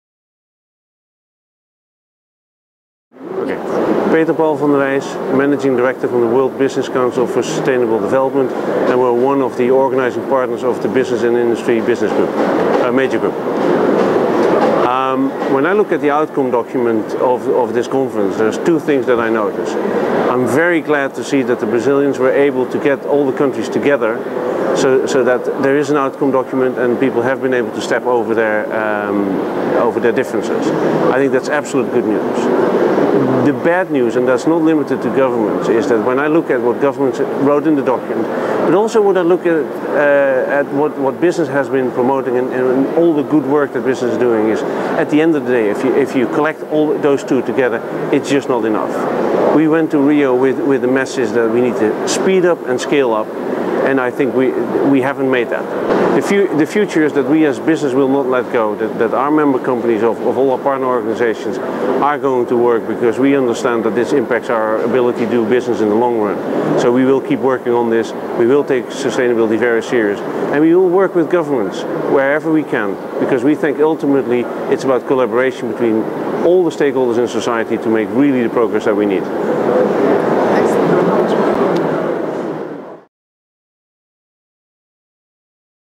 Rio+20 Major Group Business and Industry
Peter Paul van der Wijs, World Business Council for Sustainable Development (WBCSD).

O Major Group de comércio e indústria da Rio+20 elogia o texto do documento, mas expressa decepção com a falta de metas específicas para a integração do comércio e indústria verdes com todos os outros assuntos -- conduzente com uma política real de desenvolvimento auto-sustentável. Revela que a intenção do comércio e indústria é trabalhar com responsabilidade, independentemente de políticas governamentais.

Business and Industry leader Peter Paul van der Wijs praises the outcome document as a 'feat' in compromise language that looks towards the future; but, he says, business and industry are prepared to work responsibly and towards sustainable development goals, independently of any political compromise.